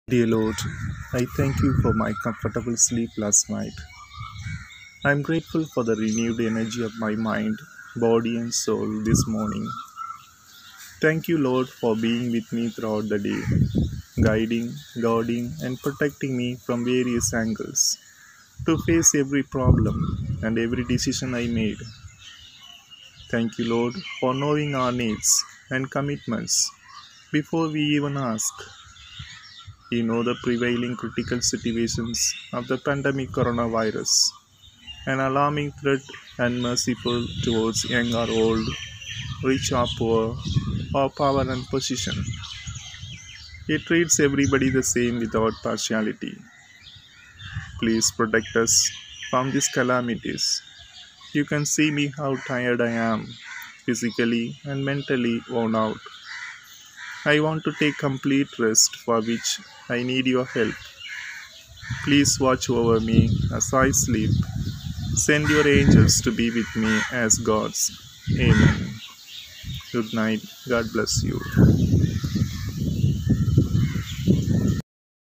Prayer for before bed time
Prayer brings happiness
Prayer brings peace of mind
Prayer helps us to understand others better
Prayer helps us to know about God and others
Before going to bed, prayer is so powerful to sleep well. God will protect all the ways.
enjoy this night prayer